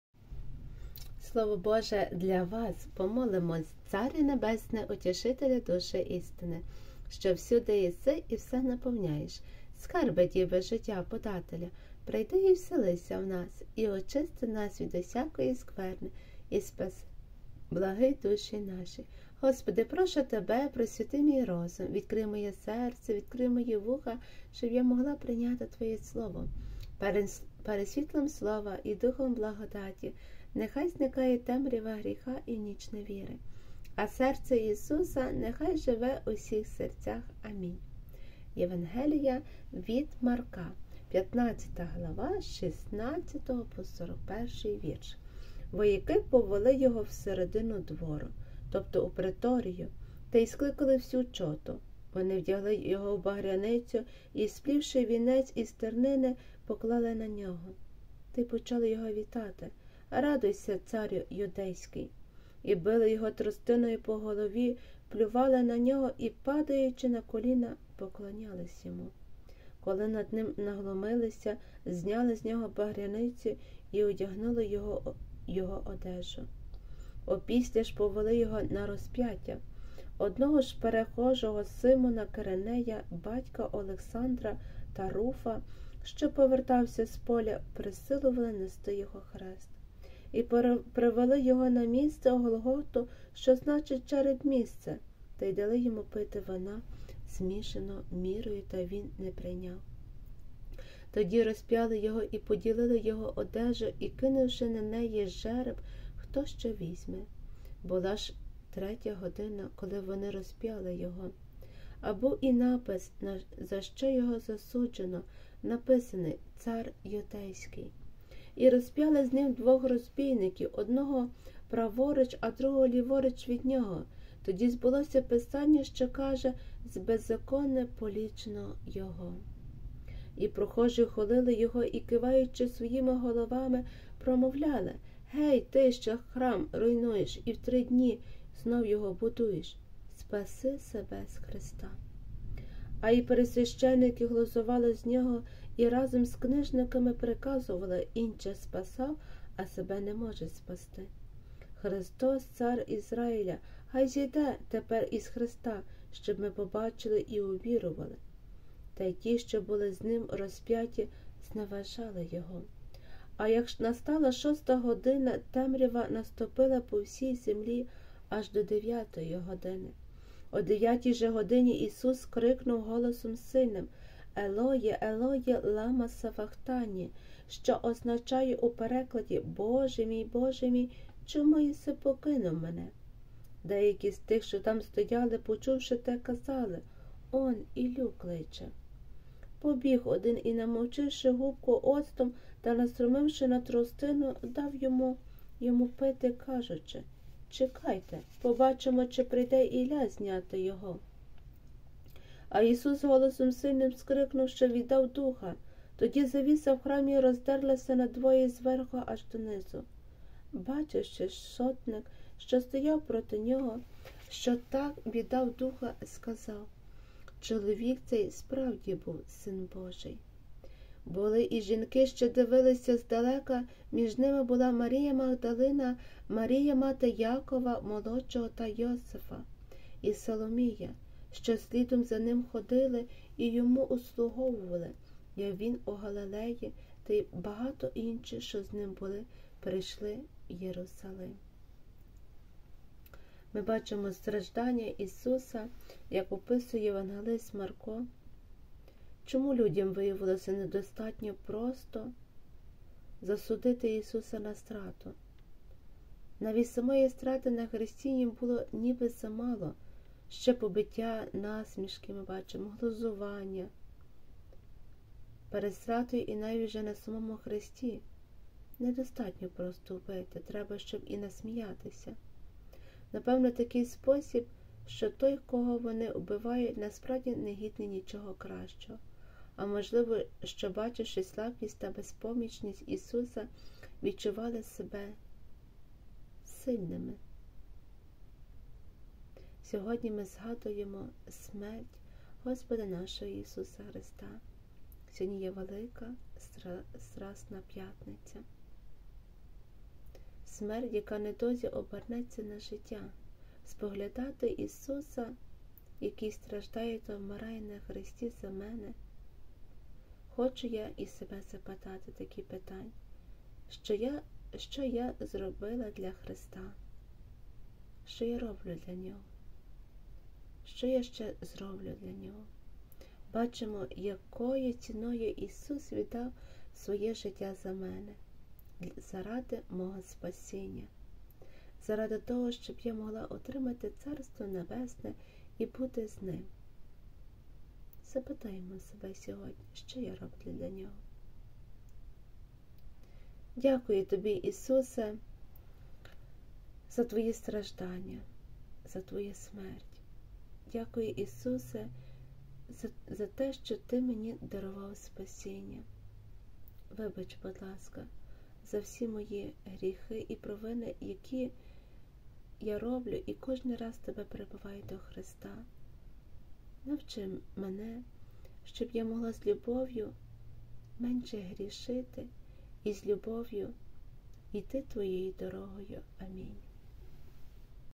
Слово Боже для вас. Євангелія від Марка 15, 16-41. Велика п'ятниця.
Сьогоднішнє Слово походить з Євангелія від Марка 15, 16-41.

16. В той час вояки повели Ісуса в середину двору, тобто у преторію, і скликали всю чоту. 17. Вони вдягли його в багряницю і, сплівши вінець із тернини, поклали на нього 18. й почали його вітати:
– Радуйся, царю юдейський!
19. І били його тростиною по голові, плювали на нього і, падаючи на коліна, кланялись йому. 20. Коли над ним наглумилися, зняли з нього багряницю й одягнули його в його одежу.
Опісля ж повели його на розп'яття. 21. І заставили одного з прохожих, Симона Киринея, батька Олександра та Руфа, що вертався з поля, нести його хрест.
22. І привели його на місце Голготу, що значить Череп-місце, 23. і дали йому пити вина, змішаного з міррою, але він не прийняв. 24. Тоді розп'яли його й поділились його одежею, кинувши на неї жереб, хто що візьме. 25. Була ж третя година, коли розп'яли його. 26. Був також напис, за що його було засуджено, на якому стояло: «Цар юдейський».
27. І розп'яли з ним двох розбійників, одного праворуч, другого ліворуч нього. 28. Тоді збулось Писання, що каже: До беззаконних зараховано його.
29. Прохожі хулили його і, киваючи головами, промовляли:
– Агов! Ти, що руйнуєш храм і в три дні знов його будуєш, 30. спаси себе, зійди з хреста!
31. Так само й первосвященики разом з книжниками, глузуючи, говорили між собою:
– Інших спасав, себе ж спасти не може! 32. Христос, цар Ізраїля, хай зійде тепер із хреста, щоб ми побачили й увірували.
І ті, що були з ним розп'яті, зневажали його.
33. А як настала шоста година, темрява наступила по всій землі аж до дев'ятої години. 34. О дев'ятій же годині Ісус скрикнув сильним голосом:
– Елої, Елої, лама савахтані? (що означає у перекладі: Боже мій, Боже мій! Чому покинув єси мене?)
35. Деякі з тих, що там стояли, почувши те, казали:
– Он, Іллю кличе!
36. Побіг один і, намочивши губку оцтом та настромивши на тростину, дав йому пити, кажучи:
– Чекайте, побачимо, чи прийде Ілля його зняти!
37. Ісус же, скрикнувши сильним голосом, віддав духа. 38. Тоді завіса в храмі роздерлася надвоє, зверху аж донизу. 39. Сотник, що стояв проти нього, бачачи, що так віддав духа, сказав:
– Чоловік цей справді був Син Божий.
40. Були там і жінки, що дивилися здалека. Між ними була Марія Магдалина, Марія, мати Якова Молодшого та Йосифа, і Саломія, 41. що слідом за ним ходили і йому услуговували, як був він у Галилеї, та й багато інших, що з ним були прийшли в Єрусалим.